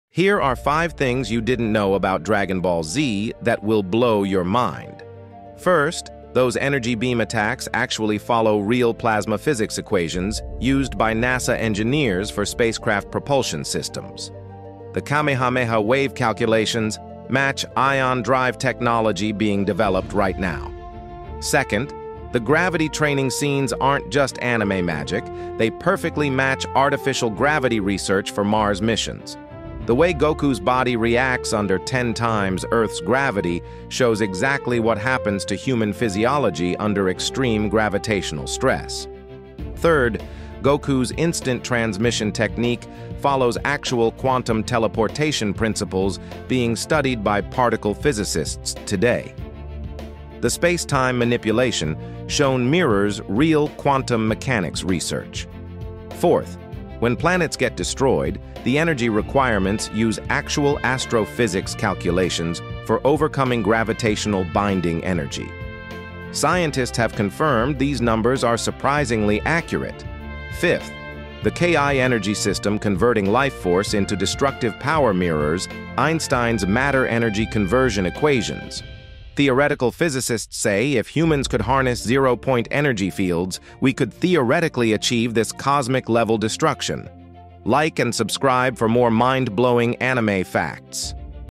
5 Dragon Ball Z Facts That Use REAL Physics (Scientists Confirmed This)
DBZ actually uses real physics equations 🤯